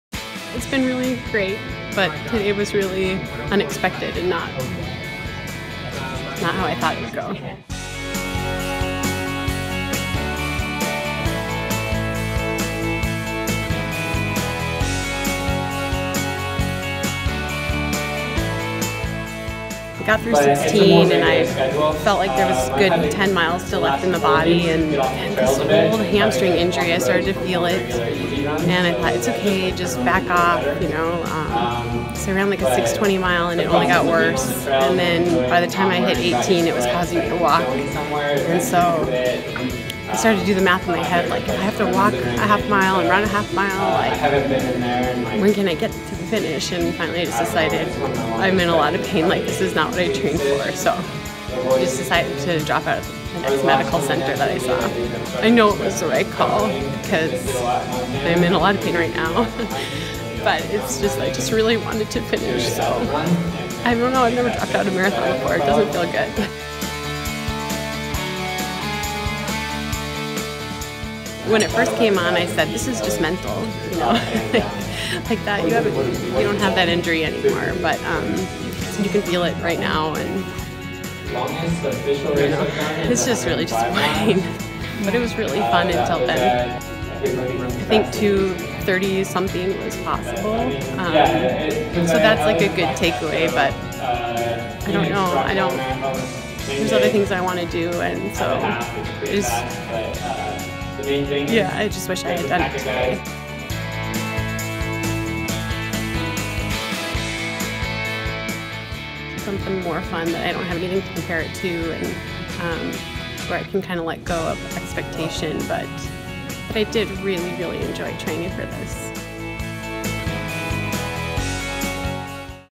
Kara Goucher Explains Why She DNF'd the 2019 Houston Marathon
“It’s been really great but today was really unexpected and not how I thought it would go.”

Kara Goucher “may be done with the roads” after dropping out of the 2019 Houston Marathon.

The 2-time Olympian hadn’t competed in a 26.2-mile race since finishing 4th at the 2016 U.S. Olympic Marathon Trials.

Kara
“I got through 16 and I felt like there was a good 10 miles left in the body. This old hamstring injury, I started to feel it, and I thought, that’s OK, just back off, so I ran a 6:20 mile and it only got worse. And then By the time I hit 18, it was causing me to walk. I started to do the math in my head, if I have to walk a half mile and run a half mile, when can I get to the finish? Finally, it was like, I’m in a lot of pain, this is not what I trained for. Just decided to drop out at the next medical center that I saw. I know it was the right call because I’m in a lot of pain right now. I just really wanted to finish. I don’t know, I’ve never dropped out of a marathon before, it doesn’t feel good.”

Goucher blames the DNF on a hamstring she first tore in 2015.

Kara
“When it first came on, I said, this is just mental, You don’t have that injury anymore. It’s just really disappointing. But it was really fun until then. I think 2:30-something was possible, so that’s a good takeaway but there’s other things I want to do. I just wish I had done it today.”

Goucher says she’s ready to “try something different” like trail running.

Kara
“Something more fun that I don’t have anything to compare it to, where I can kind of let go of expectation. But I did really, really enjoy training for this.”